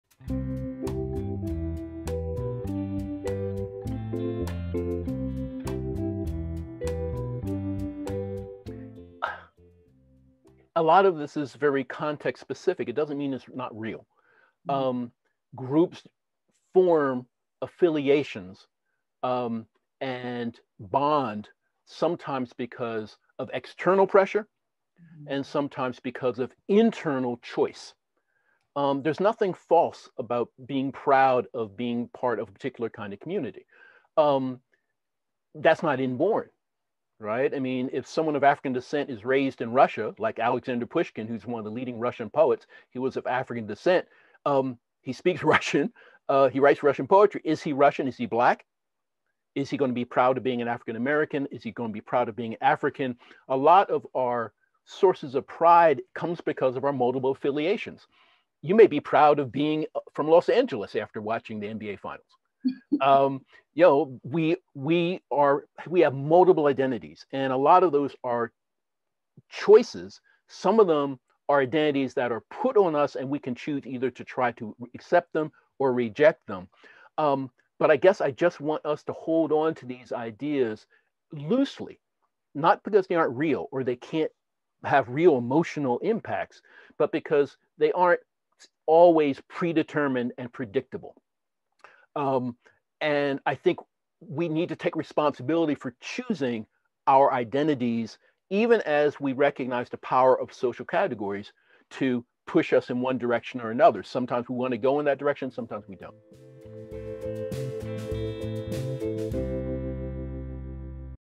Dear Iola Guest Speaker Series: Professor Yarborough on the notion of racial pride [clip]
Professor Yarborough responds to the notion of racial “pride”-that it should be understood as emerging from internal and external forces. From LitLabs Dear Iola Guest Speaker Series, October 22, 2020